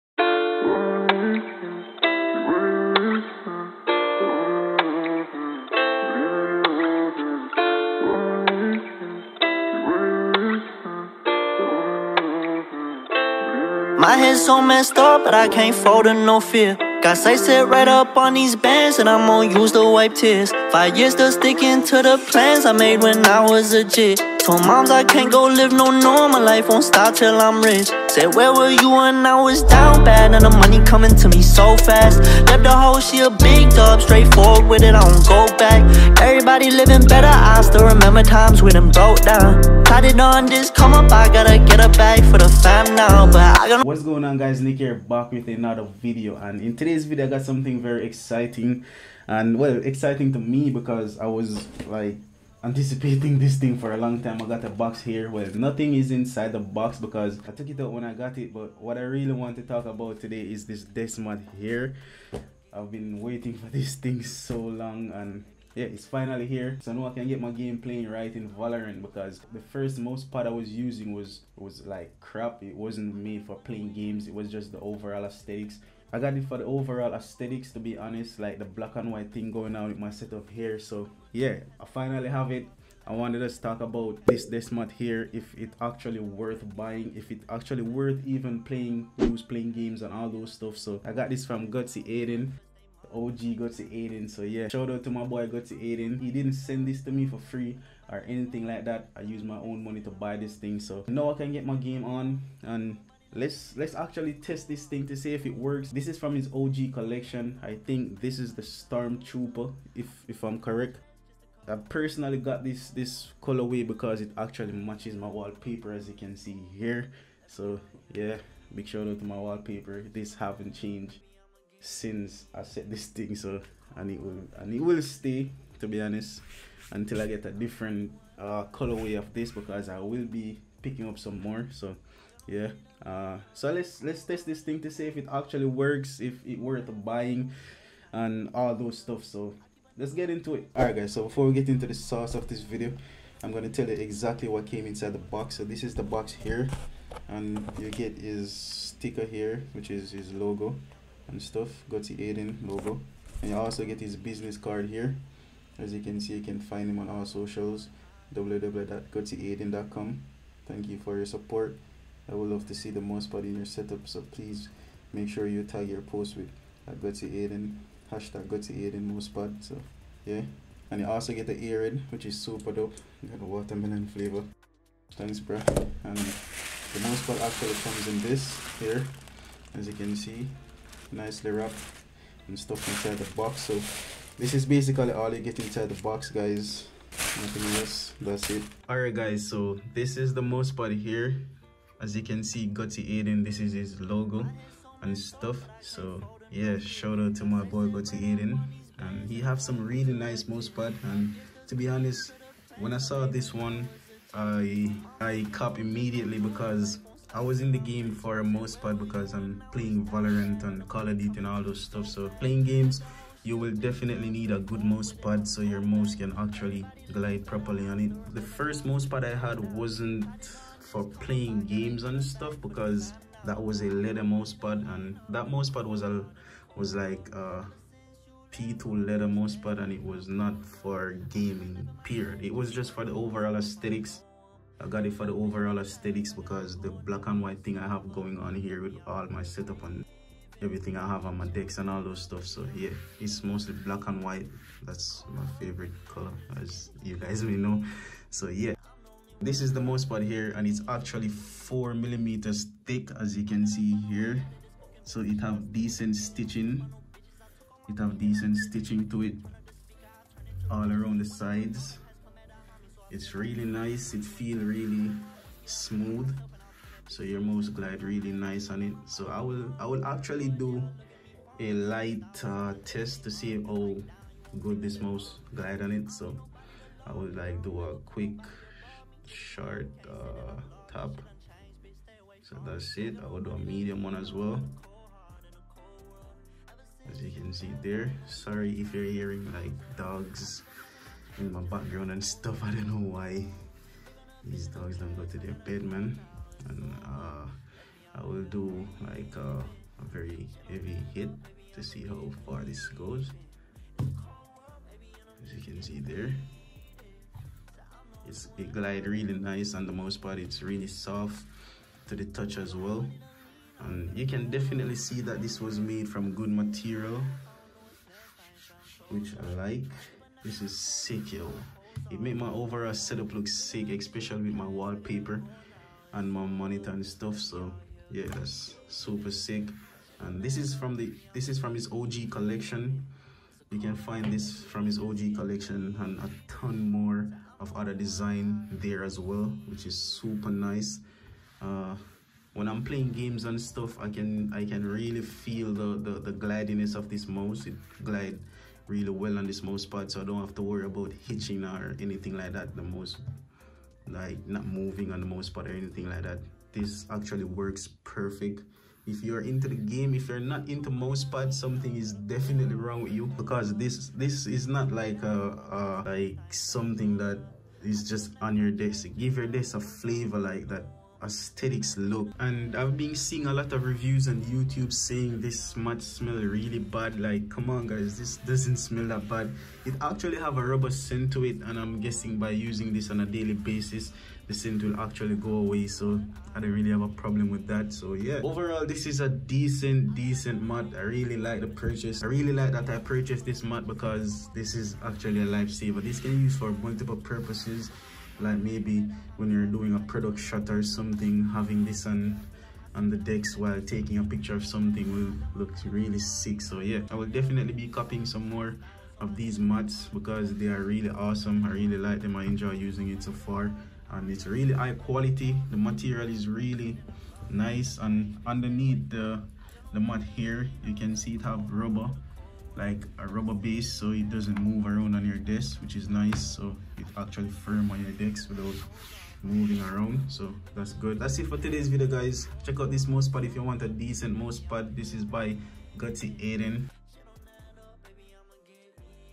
The Dream Desk| Gaming Setup & Unboxing Gutzy Aiden Mouse Pad
In todays video I decided to upgrade my gaming/ Desk setup with a nice mouse pad you can get this mouse pad at.